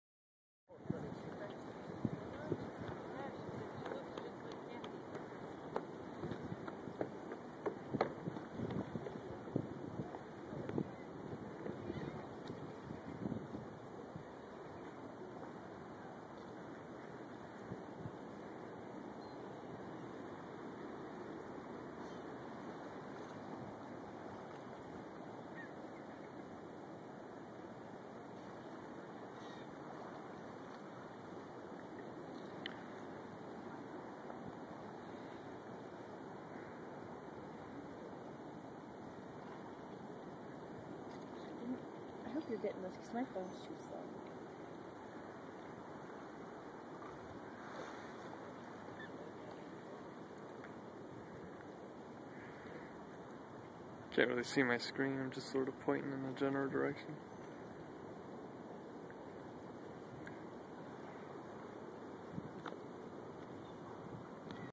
Seals off Oceanside Pier
Caught some seals munchin on some fish snacks off the Oceanside Pier in Oceanside, CA